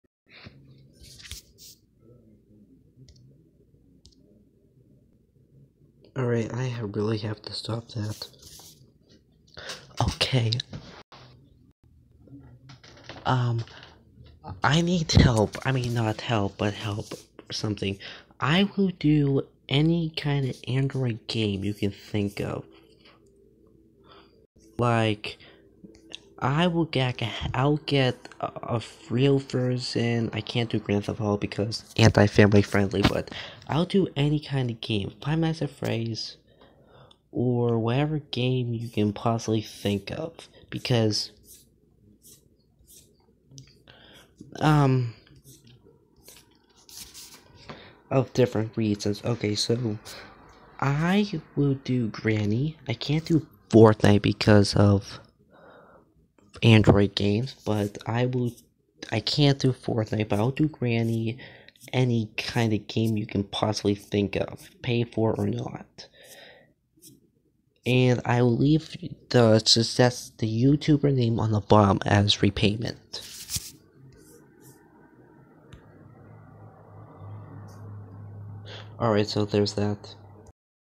HELP. I need android suggestions
No fortnite. It no work on Android.
Only Ios and apple.
Suggestions wanted
In return a shout out.